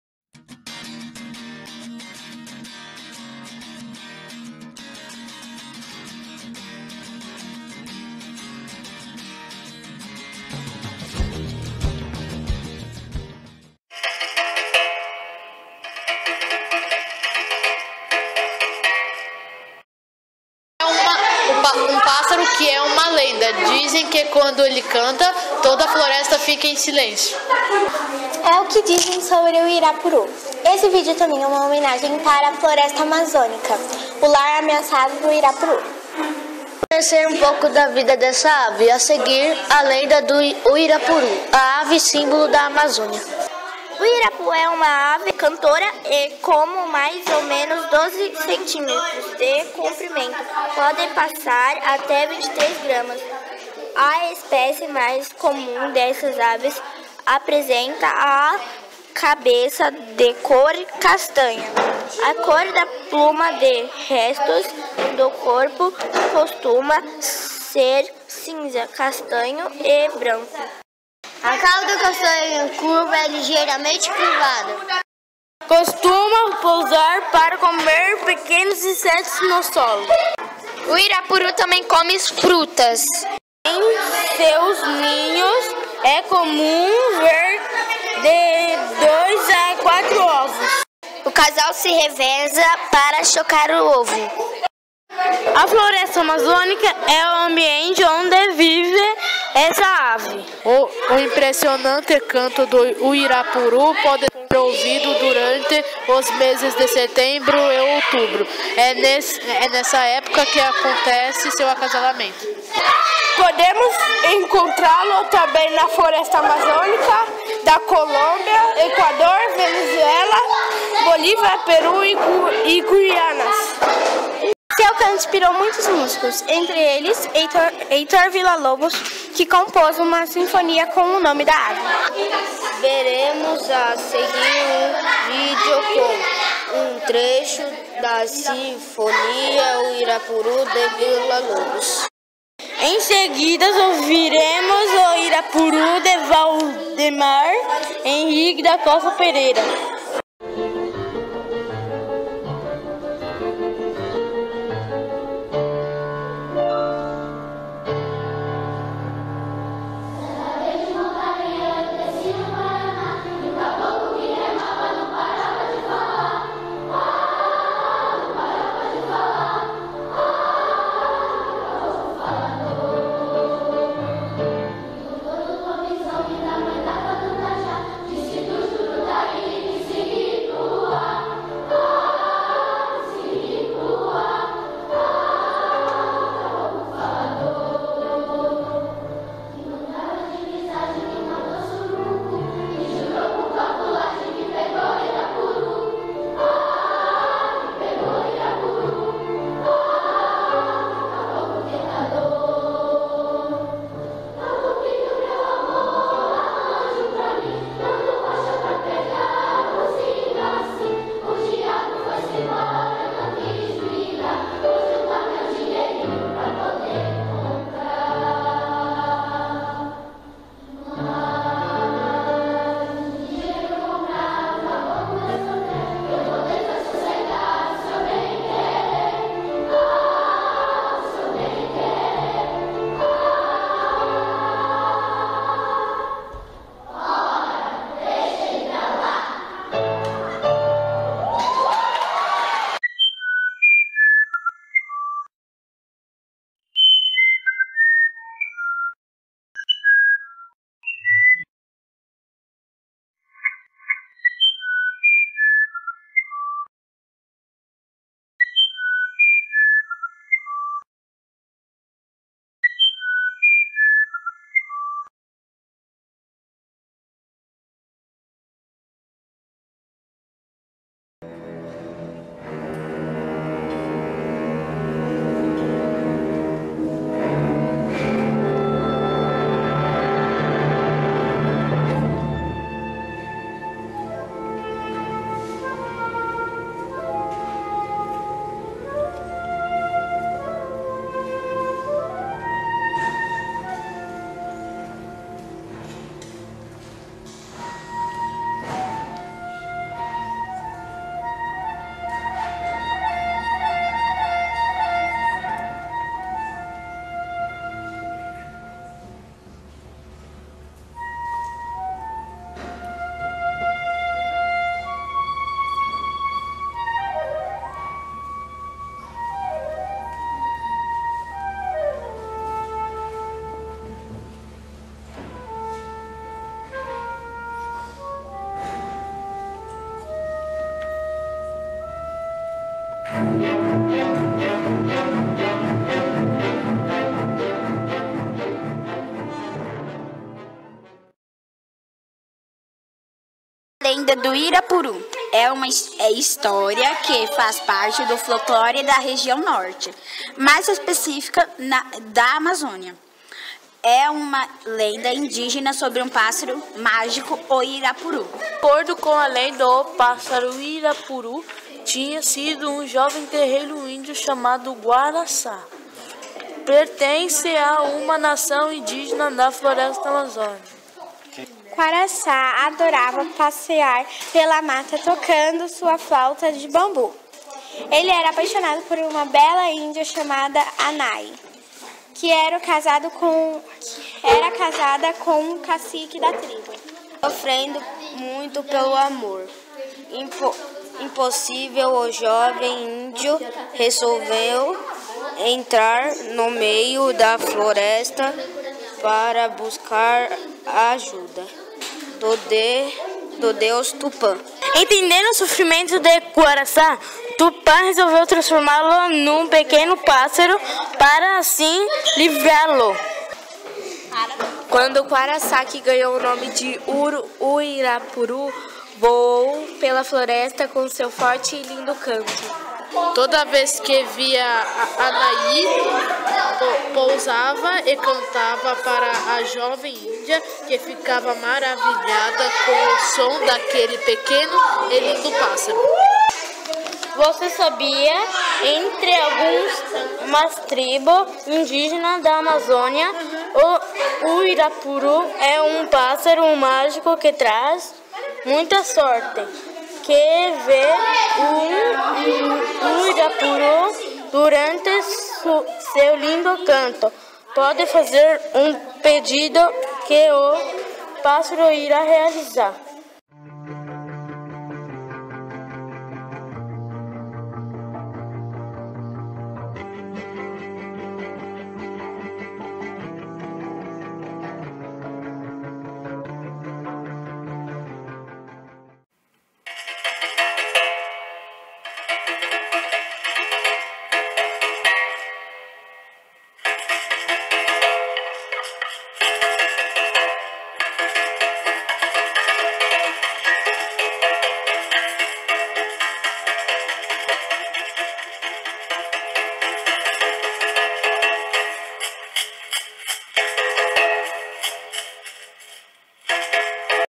Uirapuru: O Caboclo Tentador.
Documentário sobre Uirapuru realizado pelo quinto ano, período integral. A vida da ave, seu canto, músicas inspiradas no uirapuru, a lenda amazônica.